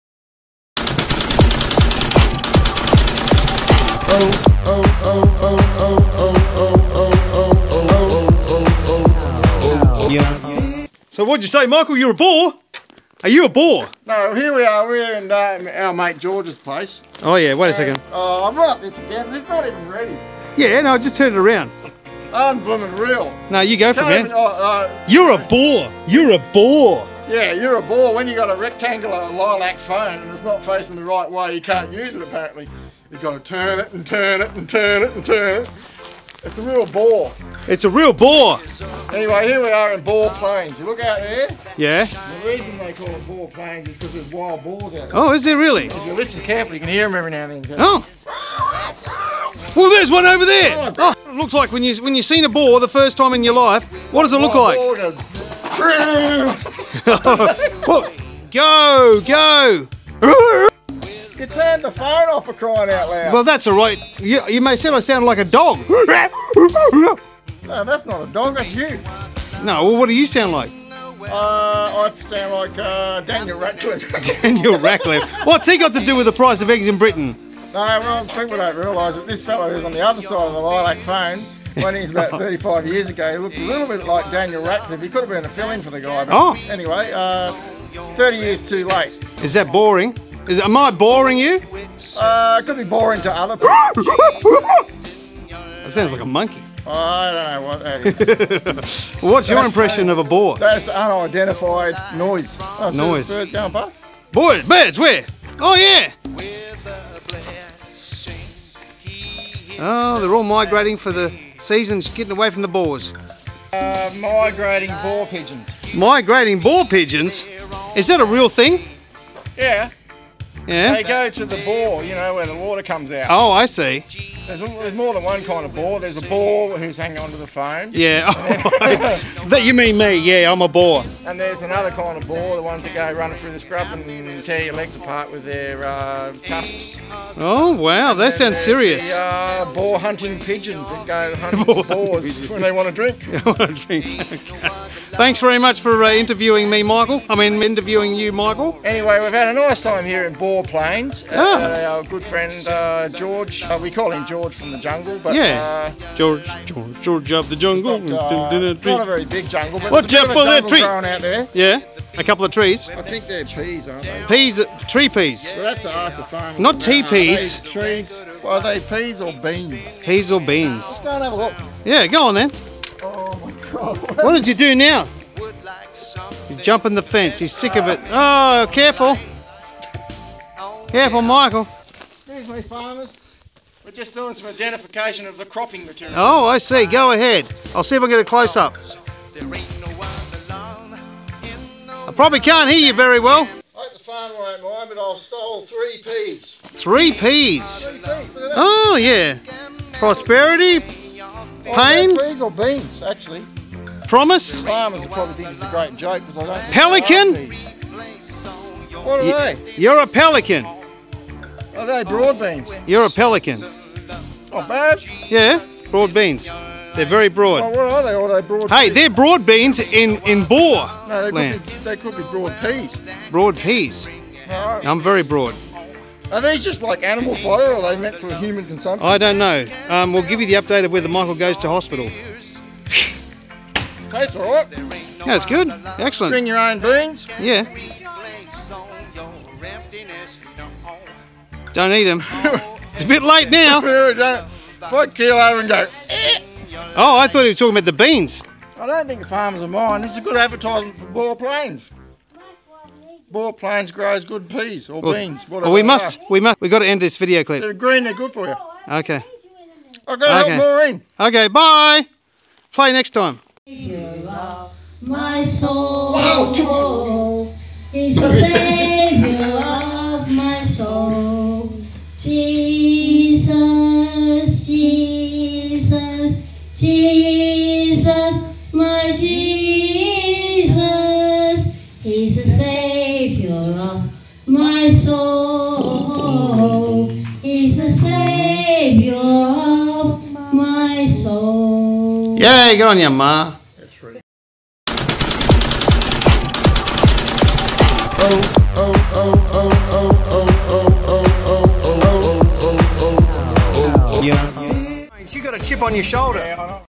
Michael is a Boar (IE: Bore) He makes noises that really sound real! (Mum Sings a Jesus Song!)
Welcome viewers of this Classic Video. Michael is giving us information concerning his favourite aniimali. the Boar, commonly referred to as a BORE! There is also the talk about BORES and the birds that search for the bores and the hidden secret staches of Bore water. Michael is a Professional Boar, I mean Bore and Im Bored. God Bless Australia!